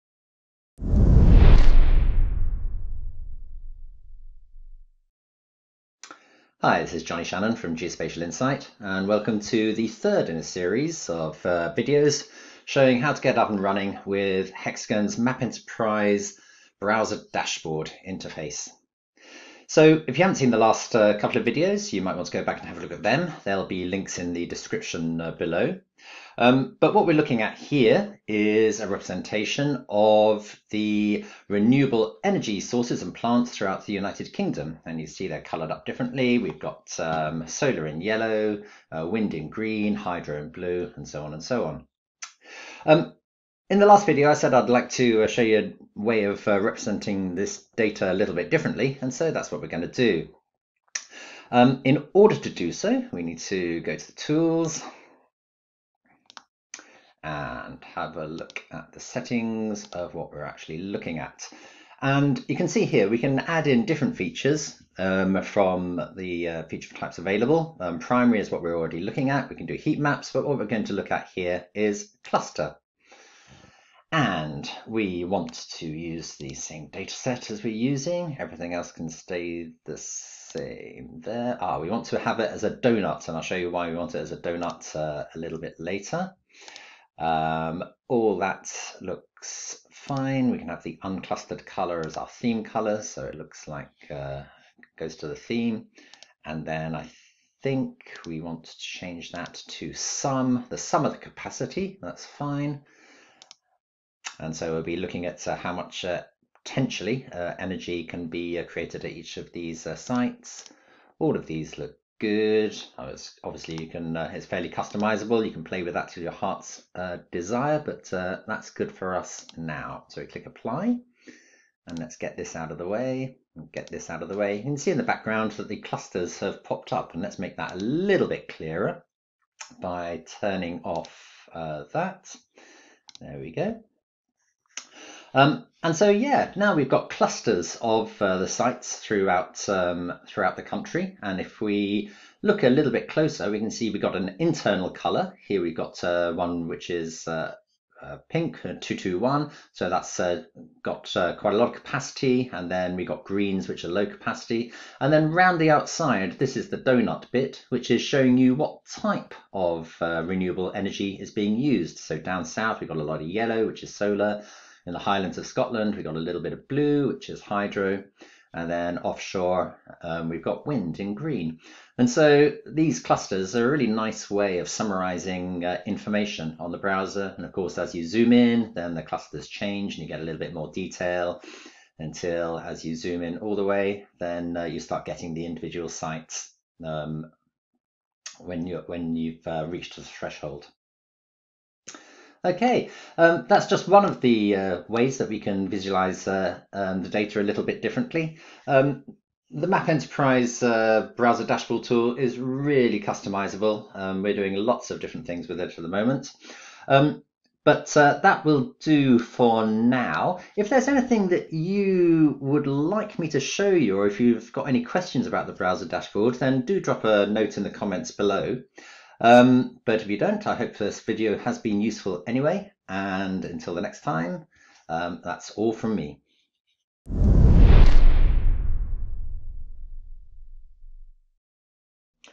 Discover the power of Dashboard - Part 3 || Simple Tutorials M.App Enterprise
A quick tutorial video explaining how to discover the functionalities of Dashboard in Hexagon Geospatial M.App Enterprise.

How to use M.App Enterprise Software || Simple Tutorials M.App Enterprise | beginners tutorial M.App Enterprise ||